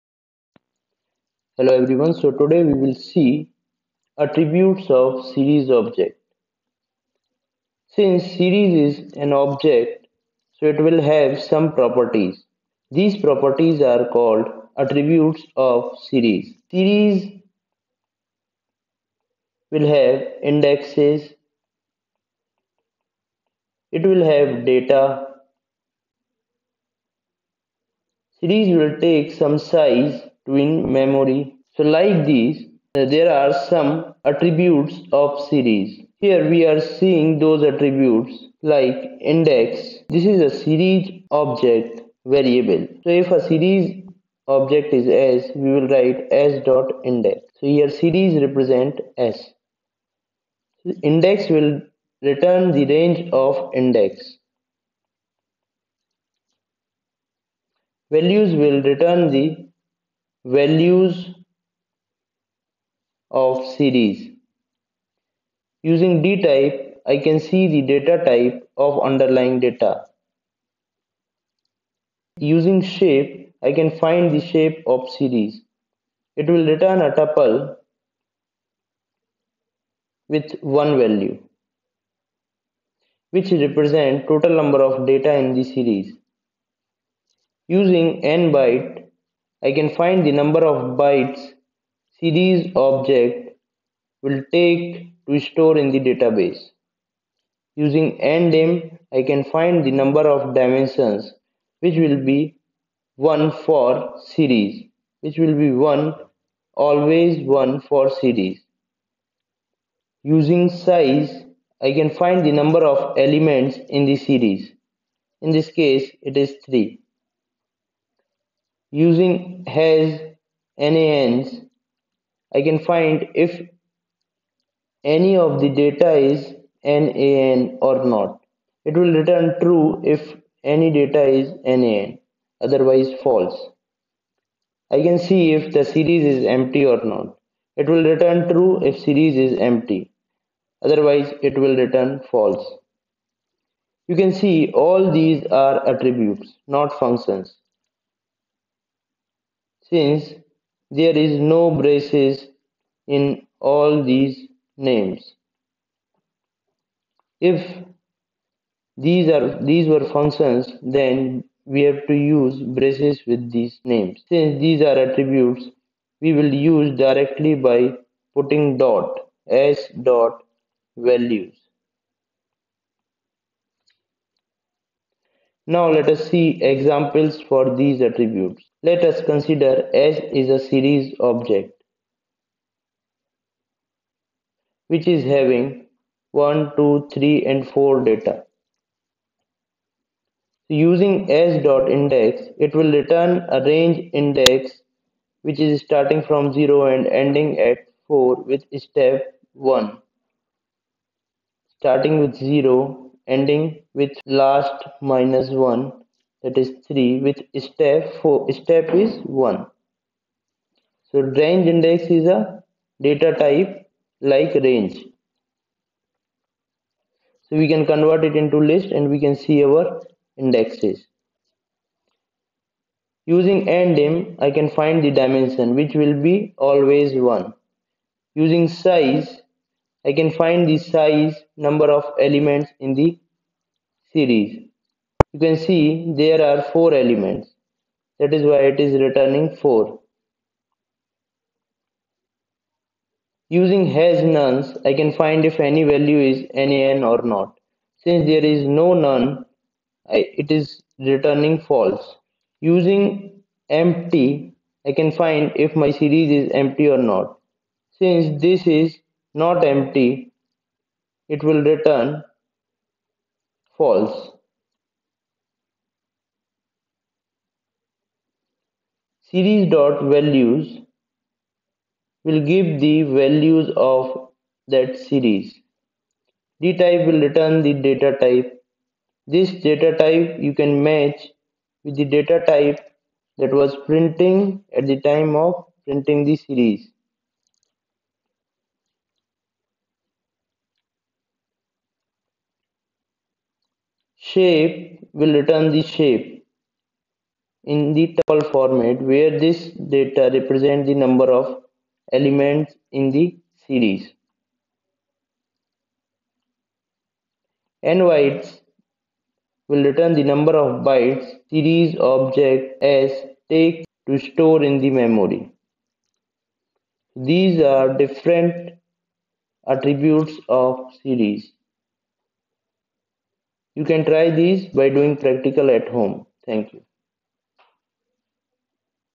Python Pandas PT-5 || Series Attributes || CLASS 12 Informatics Practices
Python Pandas Data Structure Series
Series Attributes:
-- Series.index
-- Series.values
-- Series.dtype
-- Series.shape
-- Series.nbytes
-- Series.ndim
-- Series.size
-- Series.hasnans
-- Series.empty